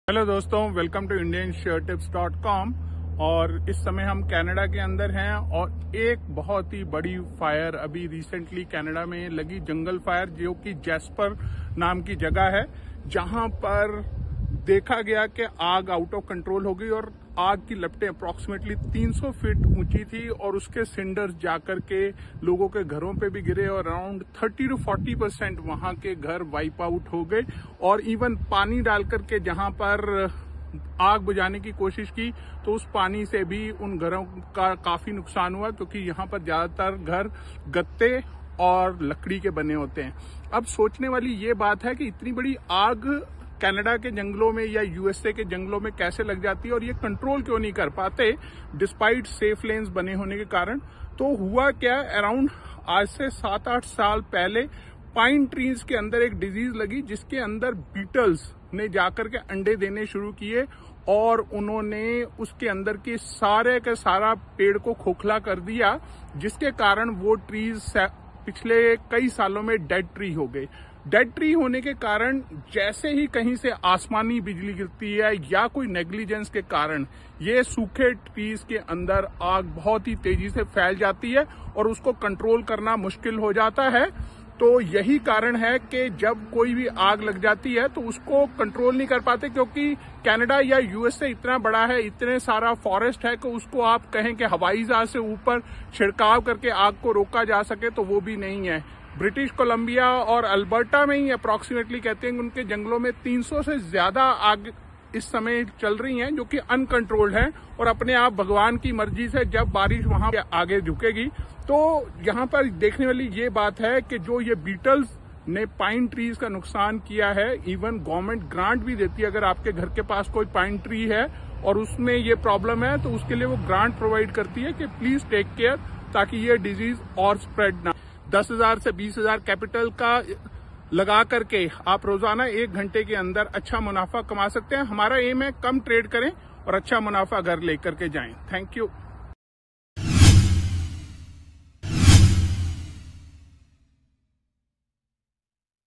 Why is Canada or USA Jungle Fire is Non Controllable?
Queries answered

Why can't Canada get their fire under control?
Does Canada do controlled burns in their forests?
What is Canada doing to prevent forest fires?
Who is responsible for forest fire management in Canada?

Alberta govt is giving relocation grant to the Jasper evacuees.

Donot forget to make profit with our bank nifty option tips and know more

Forest fires in Canada and the USA, particularly those that become uncontrollable, can often be linked to a combination of environmental and biological factors, including the impact of beetle infestations. Here are some key reasons why these fires can become so challenging to control:

### 1. **Beetle Infestations:**
   - **Mountain Pine Beetle**: One of the most significant contributors is the mountain pine beetle, which has devastated millions of acres of forest. These beetles burrow into trees, disrupting the flow of nutrients and water, eventually killing them. The dead trees become dry and highly flammable.
   - **Spruce Beetle**: Similar to the mountain pine beetle, the spruce beetle attacks spruce trees, leading to large swaths of dead and highly combustible forests.

### 2. **Climate Change:**
   - **Warmer Temperatures**: Rising temperatures due to climate change extend the fire season and create conditions that are more conducive to fires. Warmer winters have also allowed beetles to survive in greater numbers, exacerbating the problem.
   - **Drought**: Prolonged droughts dry out vegetation, making it easier for fires to start and spread rapidly.

### 3. **Forest Management Practices:**
   - **Fire Suppression**: Decades of fire suppression have led to the accumulation of combustible materials in forests. When fires do start, there is more fuel available, leading to more intense and uncontrollable fires.
   - **Lack of Thinning**: In some areas, forests have not been adequately thinned, leading to dense stands of trees that can support the rapid spread of fire.

### 4. **Weather Conditions:**
   - **High Winds**: Wind can spread fire quickly and over large areas, making it difficult to control.
   - **Dry Lightning**: Lightning strikes without accompanying rain can ignite fires in dry conditions.

### 5. **Human Activities:**
   - **Accidental Ignitions**: Human activities such as campfires, fireworks, and equipment use can accidentally start fires.
   - **Arson**: Intentional setting of fires, though less common, can also contribute to the problem.

### 6. **Geographical Challenges:**
   - **Remote Areas**: Many of the forests are in remote and rugged terrain, making it difficult for firefighting crews and equipment to access and contain fires quickly.

### 7. **Ecological Factors:**
   - **Fuel Types**: Different types of vegetation and trees burn differently. For example, resinous trees like pines can burn hotter and faster.
   - **Fire Behavior**: Some ecosystems have evolved with fire as a natural part of their cycle. In these areas, fire can be beneficial, but the conditions created by beetle infestations and climate change can turn these natural fires into uncontrollable events.

### Mitigation and Control Efforts:
   - **Forest Management**: Implementing better forest management practices, including controlled burns and thinning, can help reduce the fuel load.
   - **Monitoring and Early Detection**: Improved monitoring systems for early detection of fires and beetle infestations can help in taking timely actions.
   - **Research and Technology**: Investing in research and technology to understand fire behavior and improve firefighting techniques.

Addressing the underlying causes, such as beetle infestations and climate change, along with improving forest management practices, is crucial in making these fires more controllable in the future.